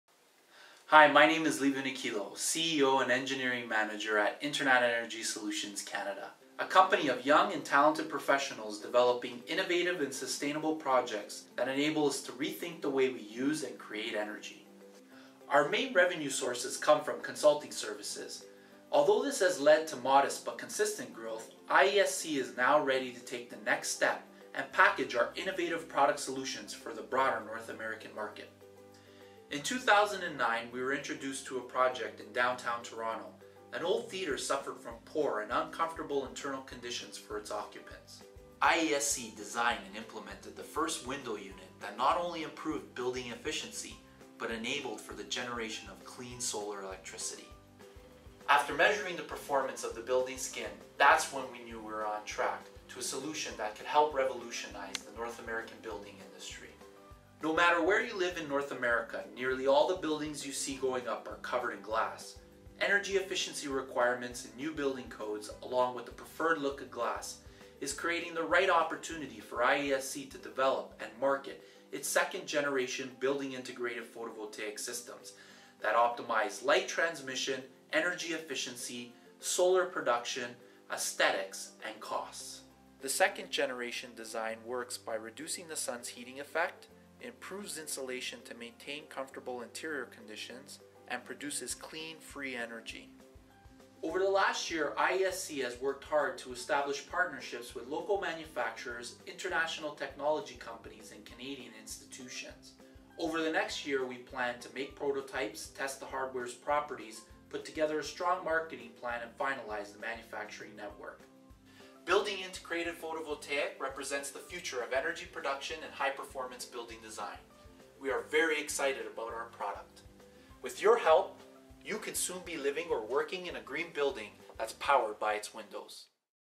Building-Integrated Photovoltaic (BIPV) Glazing for Building Envelopes
IESC CEO and Engineering Manager Livio Nichilo explains the ongoing R+D project at IESC to design and commercialize an innovative BIPV glazing unit for building envelopes that will help buildings save energy while generating clean electricity.